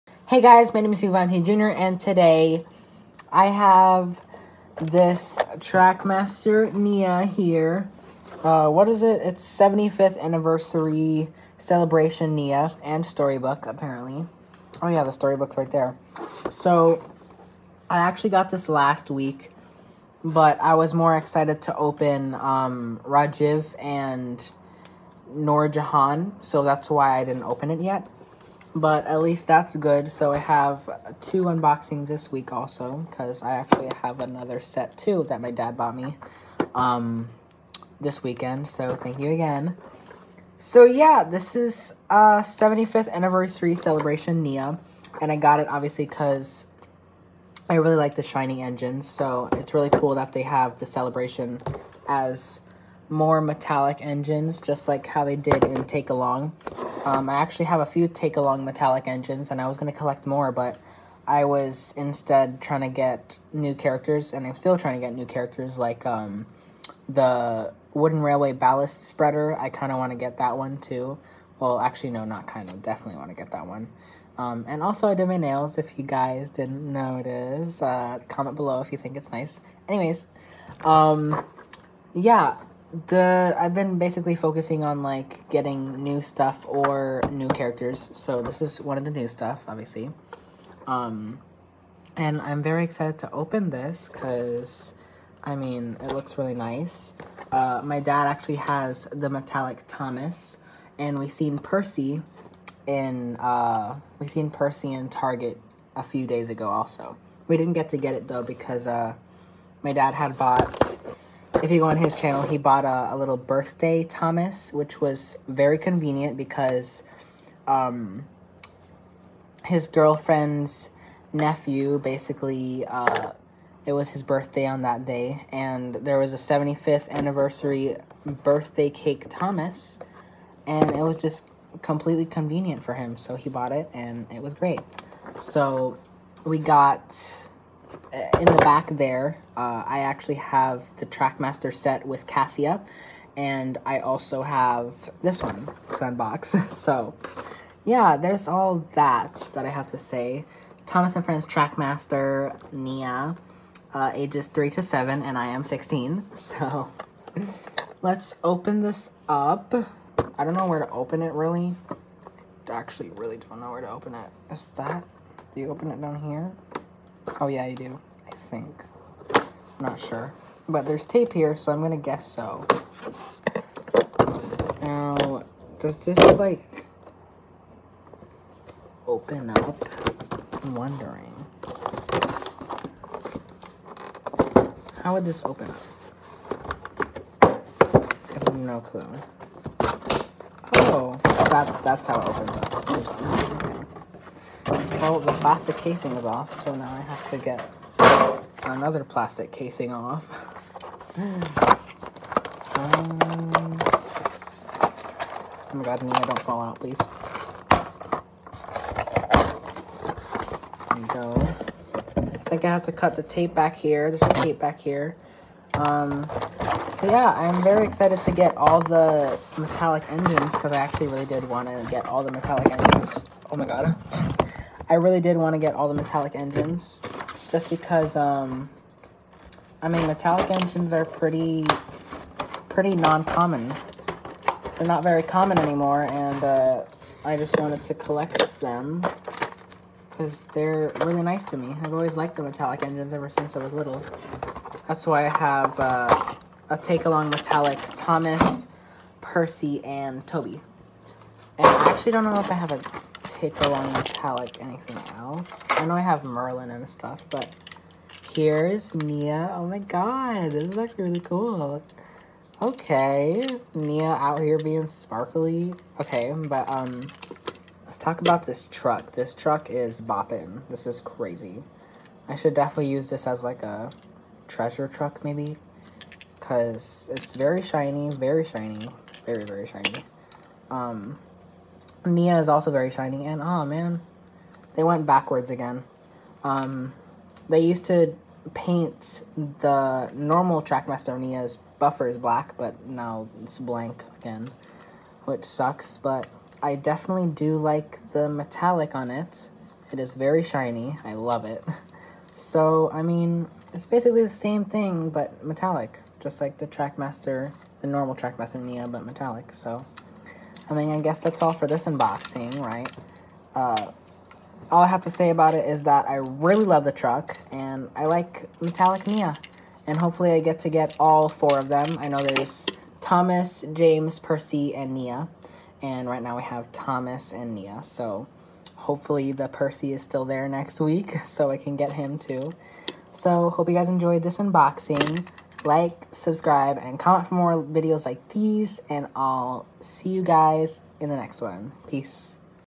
Trackmaster 75th Anniversary Nia Unboxing!
Finally I unbox my 75th Anniversary Nia after a week of waiting! Hope you enjoy this unboxing.